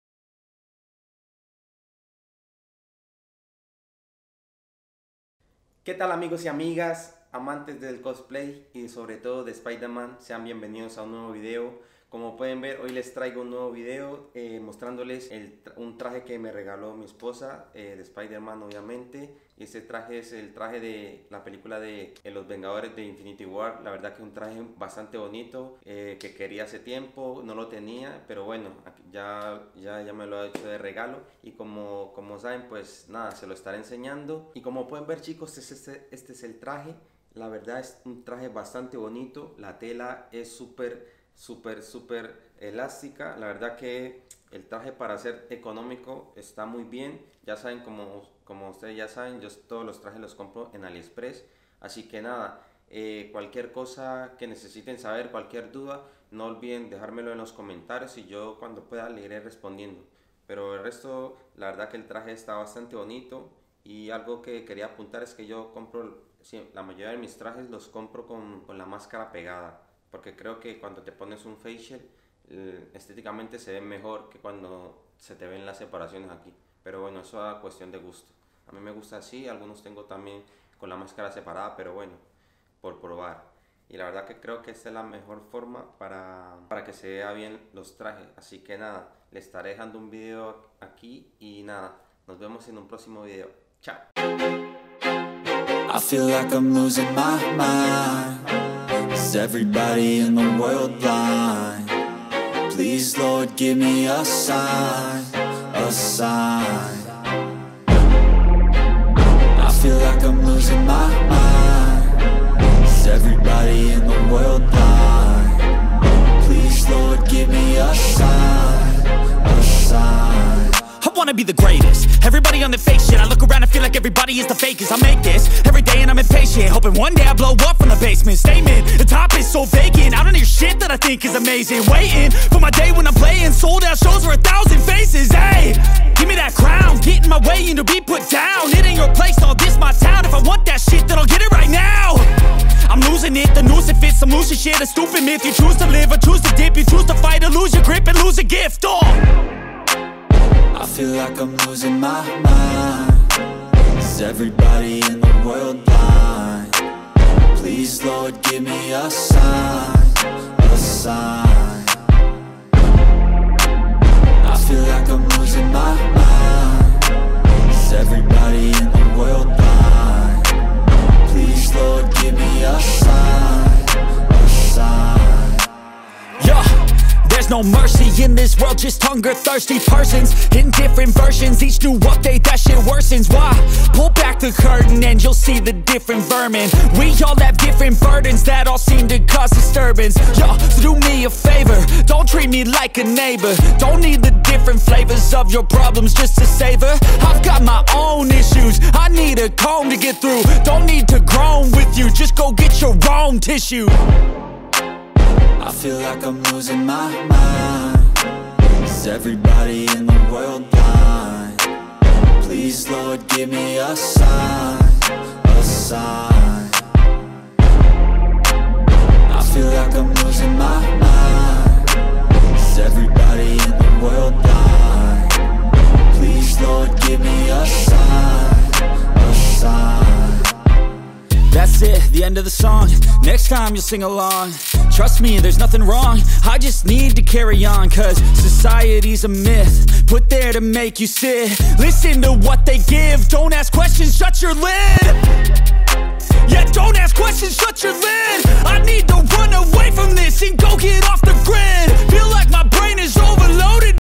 vale la pena traje iron spider de aliexpress review
Amigos hoy les comparto este traje de spiderman el que uso en la película de los vengadores infinity war, es un traje con mucha tecnología y como saben lo conseguí de aliexpress! Ha sido un regalo de navidad y les quería mostrar espero les guste y para todo aquel que quiere comprar un traje sea su primero o uno mas para su colección quizás le sea util este video a tomar una decisión de comprar en aliexpress